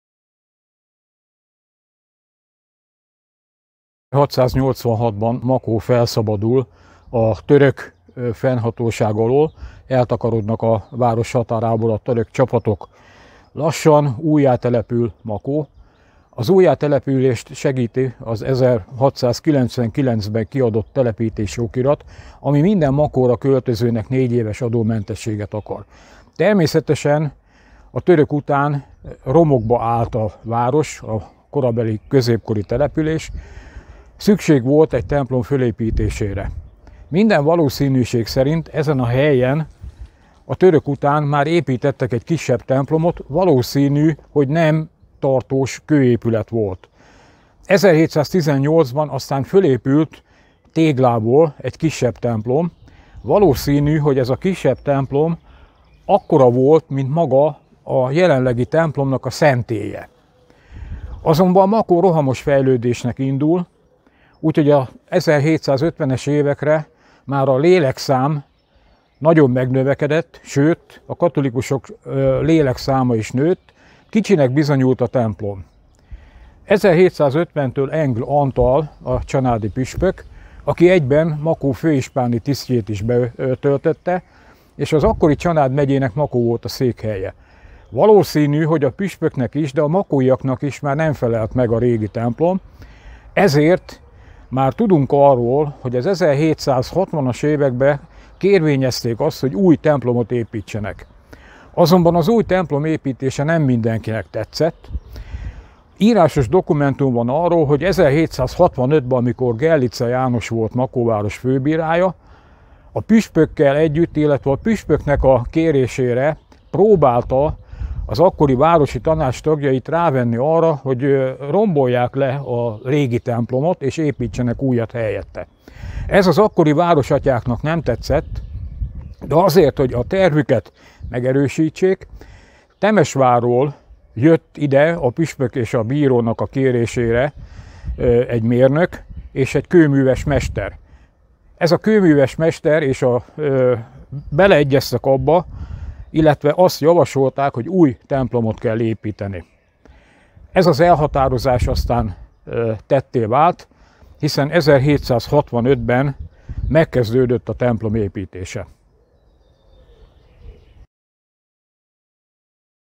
ÉRTÉK-TÁR(GY) - Gilicze János levéltáros (templom építéstörténet 1.)
A Szent István Király Plébániatemplom rejtett kincsei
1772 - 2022